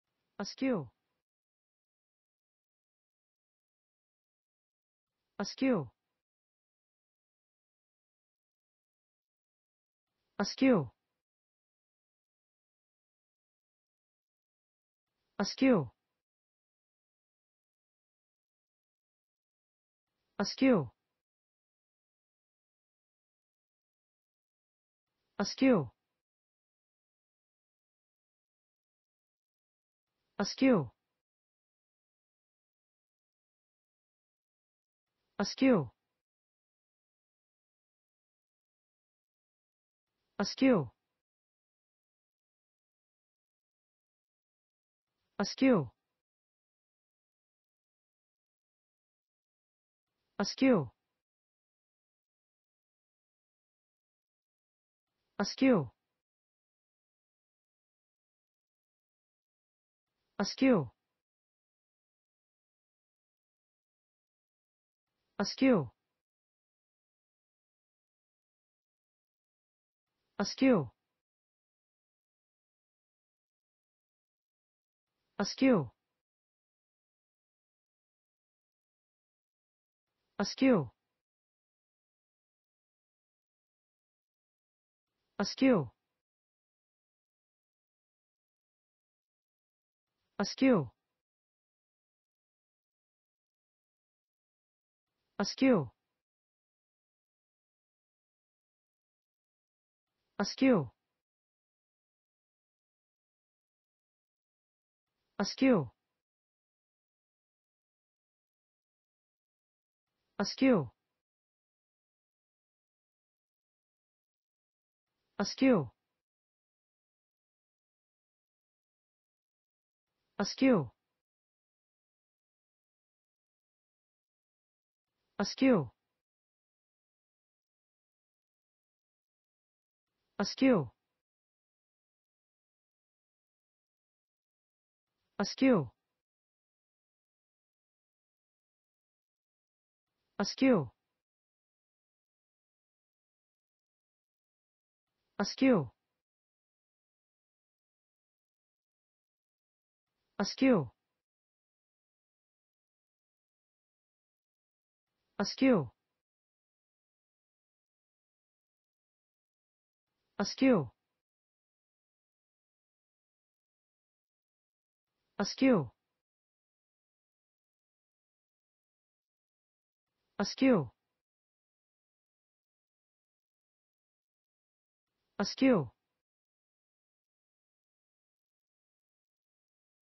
Aascu: How to pronounce Aascu in english (correct!).Start with A. Learn from me.
Follow me!Learn from me how to pronounce the word Aascu in english.This video shows you how to pronounce the word Aascu in english.

Aascu